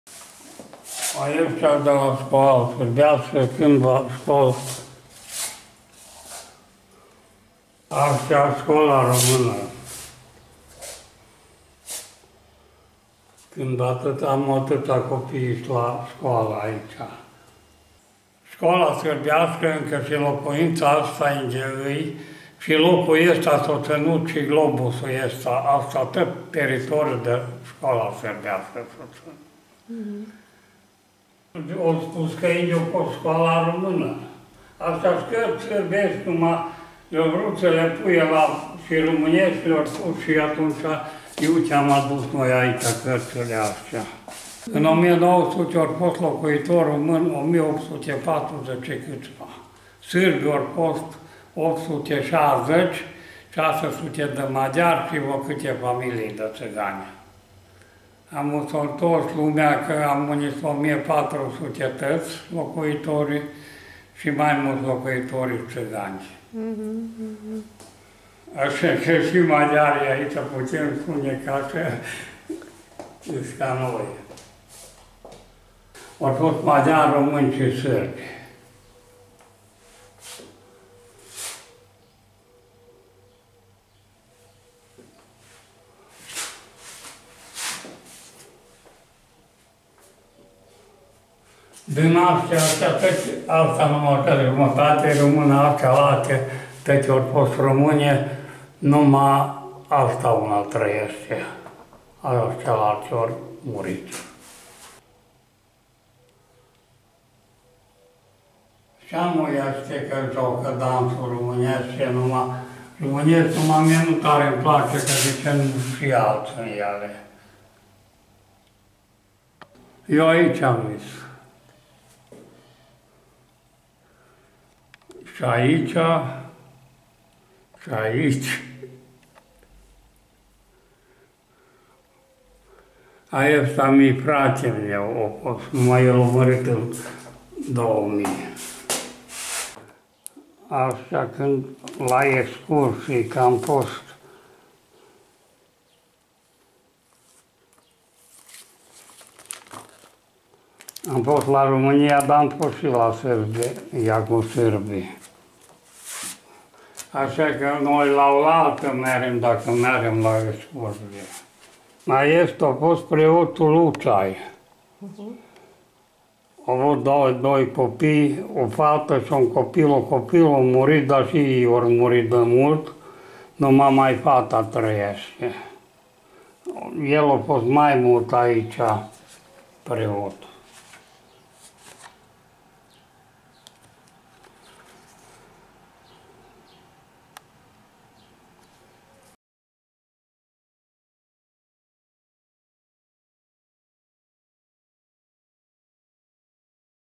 Čanad / Cenadul Unguresc/ Magyarcsanád, Mađarska, 23.08.2019
Manjinska samouprava Srba u Čanadu / Autoguvernarea minoritară română Cenadul Unguresc
Terenska istraživanja Srba u Mađarskoj u organizaciji Srpskog instituta, Budimpešta, Szerb Intézet, Budapest
Snimak i montaža: BIljana Sikimić
Digitalni arhiv Balkanološkog instituta SANU, Beograd